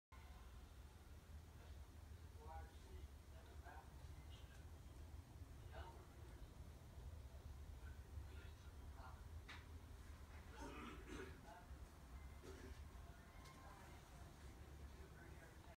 CoolCat Art Mobile by AtomicMobiles.com - midcentury modern retro mobiles
Atomic mid century modern Calder-inspired art sculpture. Custom handmade when you order by Debra Ann in her studio in Los Angeles, California. CoolCat style. Over 30 beautiful colors and variety of sizes to choose from, perfect for your home or office decor. Go to AtomicMobiles.com to see more!